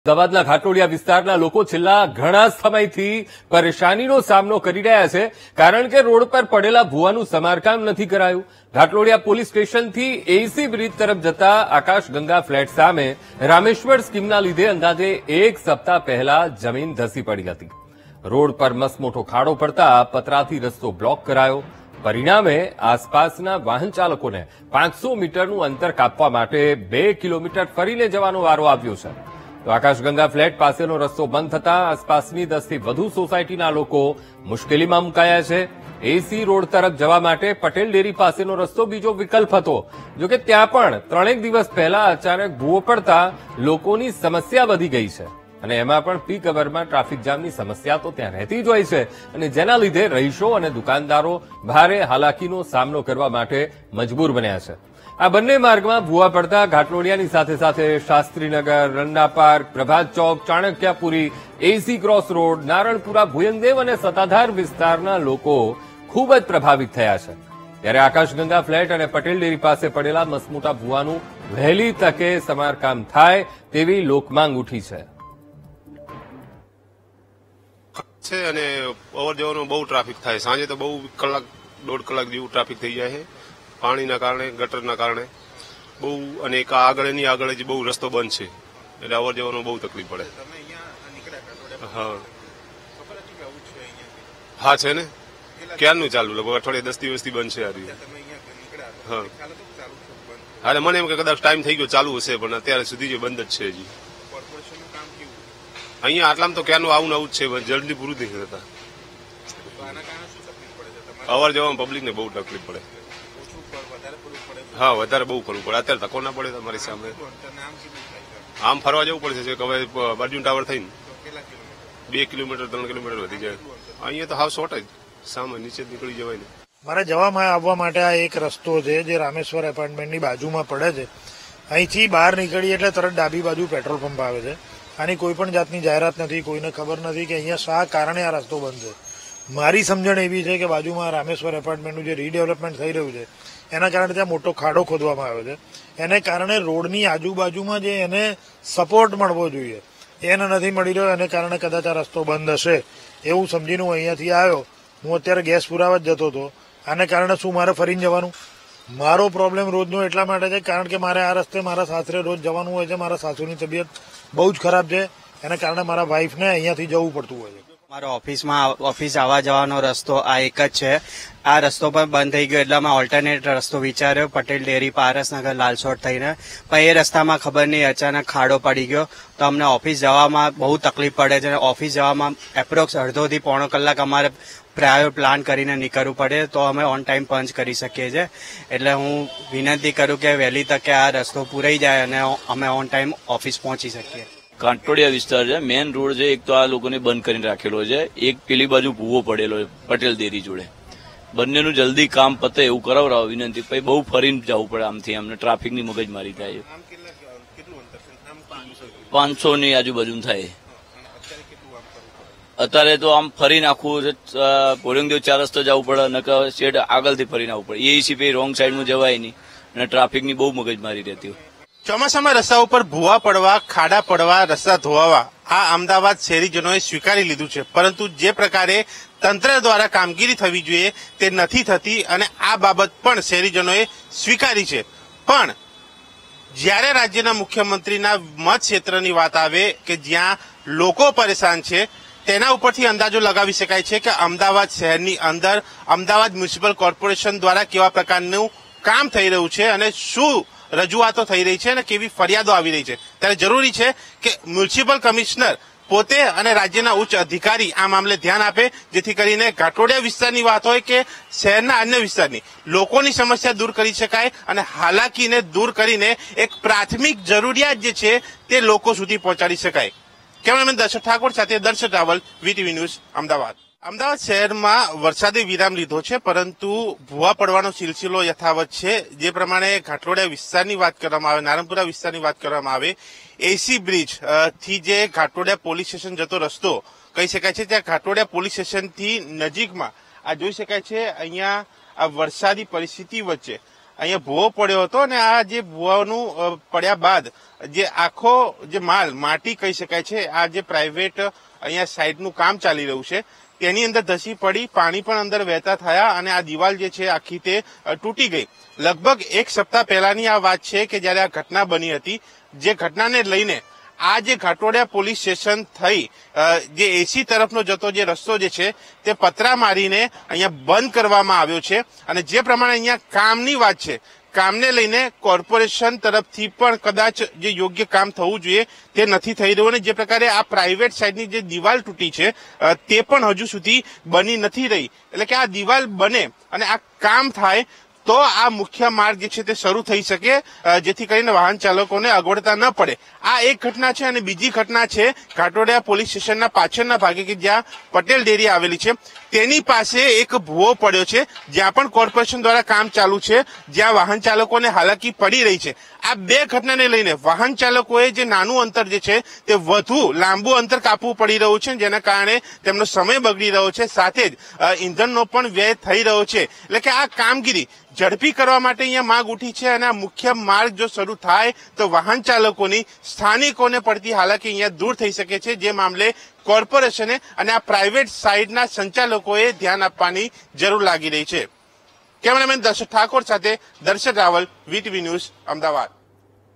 Ahmedabadના ઘાટલોડિયા વિસ્તારના લોકો મૂશ્કેલીમાં મૂકાયા, સપ્તાહ પહેલા પડેલા ભૂવાનું નથી થયું સમારકામ
અમદાવાદના ઘાટલોડિયા વિસ્તારના લોકો છેલ્લા ઘણા સમયથી પરેશાન થયા છે.. કારણ કે, રોડ પર પડેલા ભૂવાનું સમારકામ નથી કરાયું.. ઘાટલોડિયા પોલીસ સ્ટેશનથી AEC બ્રિજ તરફ જતા આકાશ ગંગા ફ્લેટ સામે રામેશ્વર સ્કીમના લીધે અંદાજે એક સપ્તાહ પહેલા જમીન ધસી પડી હતી.. રોડ પર મસમોટો ખાડો પડતા પતરાથી રસ્તો બ્લોક કરાયો.. પરિણામે. આસપાસના વાહન ચાલકોને 500 મીટરનું અંતર કાપવા માટે 2 કિલો મીટર ફરીને જવાનો વારો આવ્યો છે.. આકાશગંગા ફ્લેટ પાસેનો રસ્તો બંધ થતા આસપાસની 10થી વધુ સોસાયટીના લોકો મૂશ્કેલીમાં મૂકાયા છે.. એઈસી રોડ તરફ જવા માટે પટેલ ડેરી પાસેનો રસ્તો બીજો વિકલ્પ હતો.. જો કે ત્યાં પણ ત્રણેક દિવસ પહેલા અચાનક ભૂવો પડતા લોકોની સમસ્યા વધી ગઈ છે.. તેમાં પણ પીક-અવરમાં ટ્રાફિકજામની સમસ્યા થઈ જાય છે.. જેના લીધે રહીશો અને દુકાનદારો ભારે હાલાકીનો સામનો કરવા મજબૂર બન્યા છે.. આ બંને માર્ગમાં ભૂવા પડતા ઘાટલોડિયાની સાથે સાથે શાસ્ત્રીનગર, રન્નાપાર્ક, પ્રભાતચોક, ચાણક્યપુરી, એઈસી ક્રોસ રોડ, નારણપુરા, ભુયંગદેવ અને સત્તાધાર વિસ્તારના લોકો ખૂબ પ્રભાવિત થયા છે.. ત્યારે આકાશગંગા ફ્લેટ અને પટેલ ડેરી પાસે પડેલા મસમોટા ભૂવાનું વહેલી તકે સમારકામ થાય તેવી લોકમાંગ ઉઠી છે..